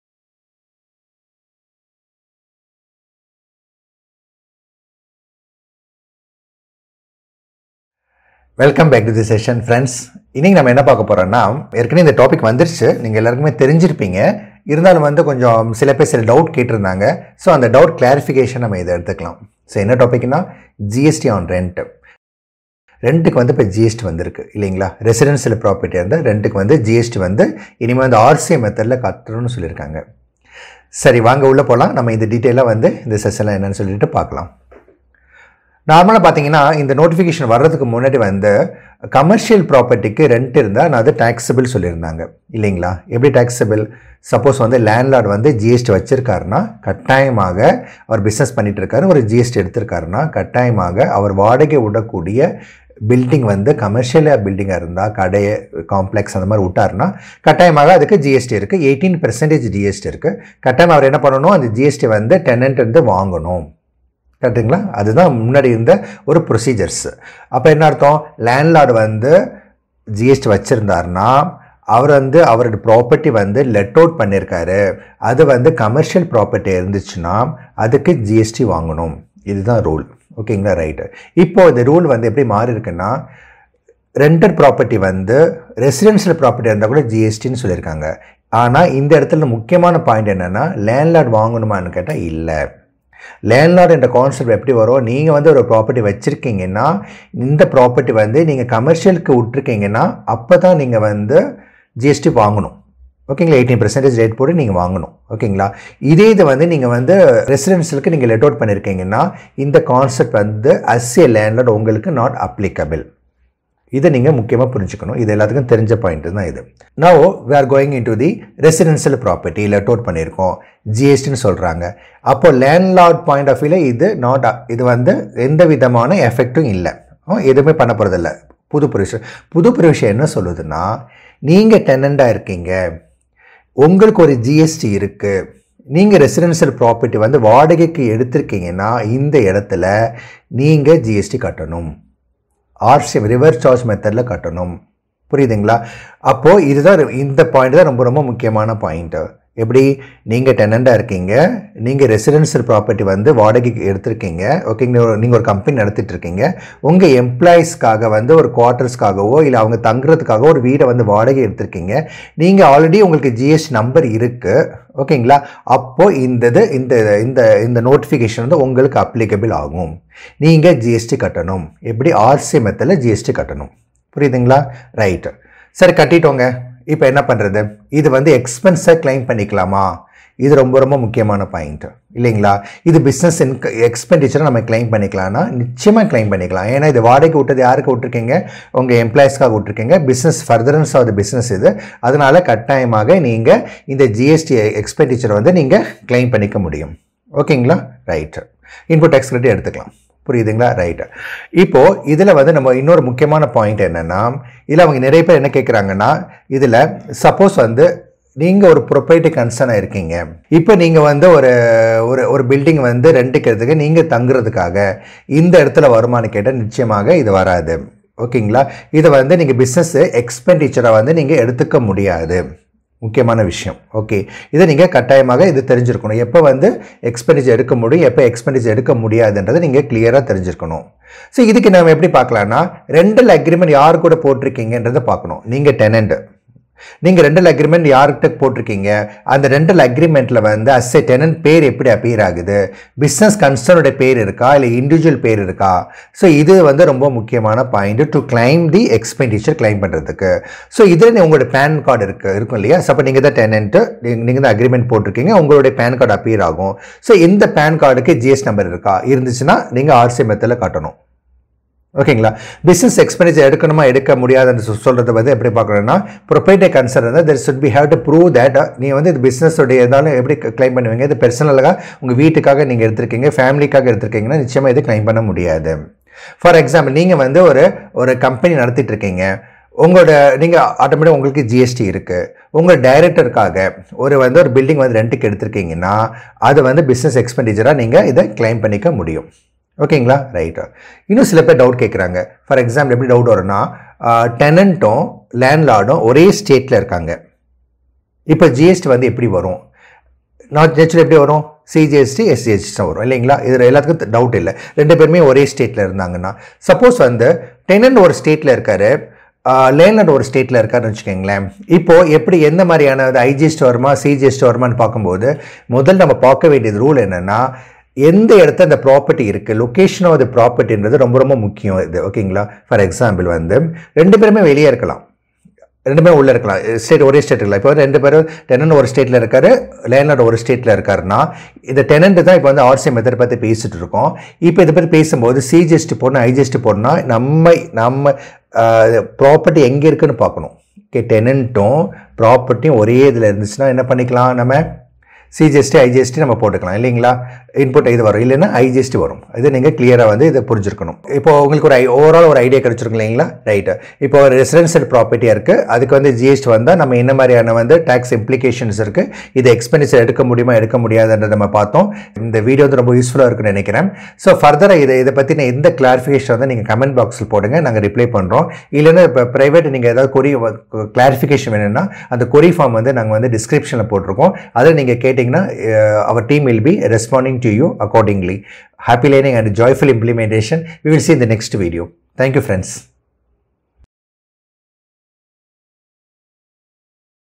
GST on Rent Residential Property | Tamil | GST payable by Tenant | தமிழ்
Latest GST notification on GST on the Rent is implementable with immediate effect. The detailed discussion of GST on Rent paid by the Tenant is covered

GST on Rent